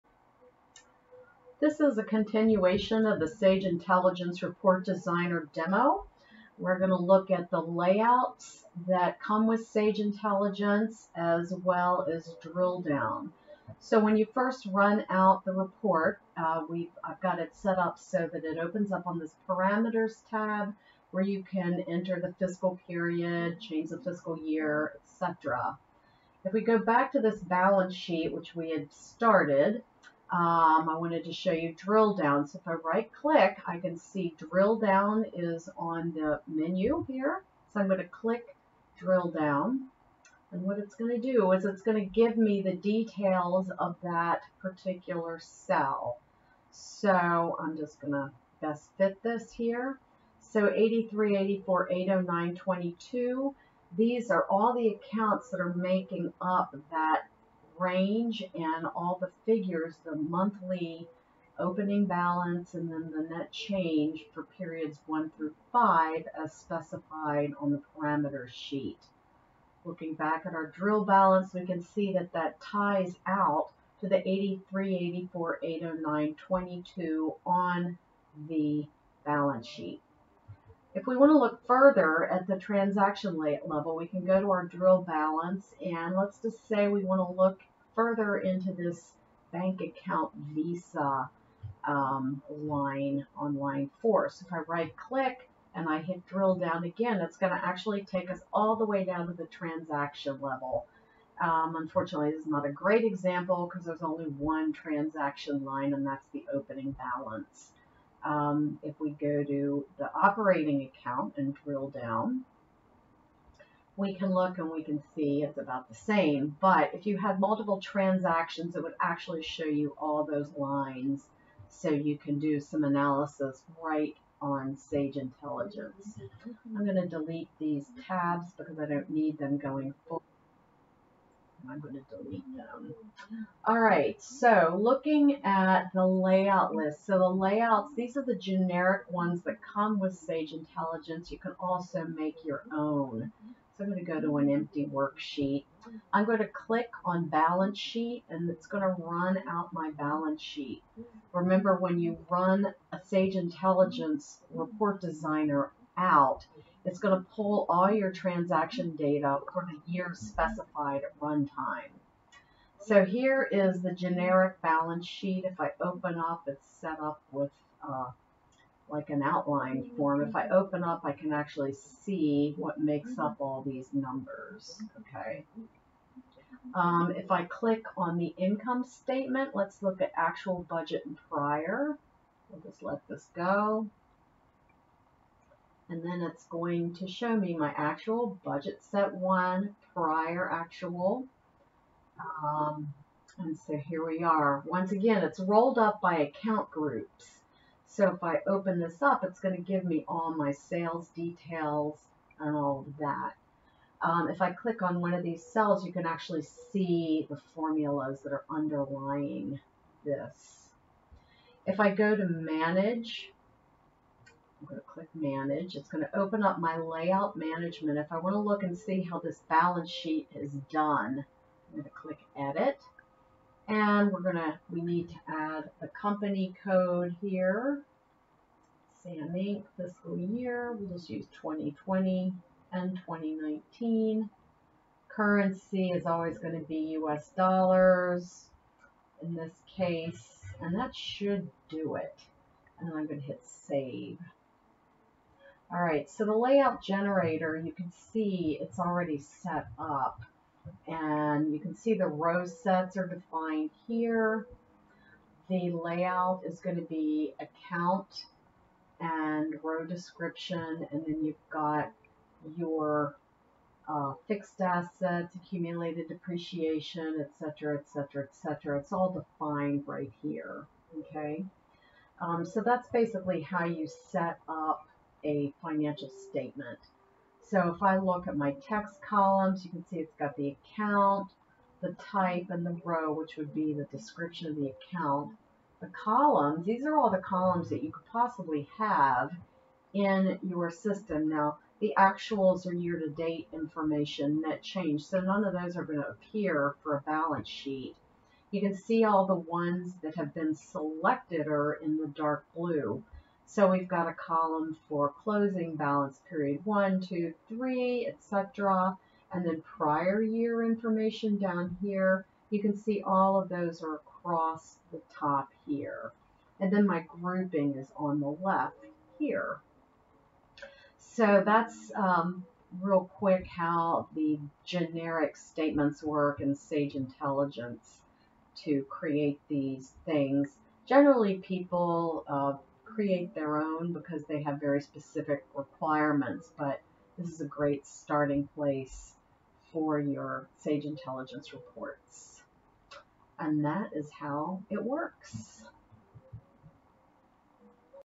SI Report Designer Layouts and Drill Down
Sage Intelligence demo - report designer, using drill down and layout generator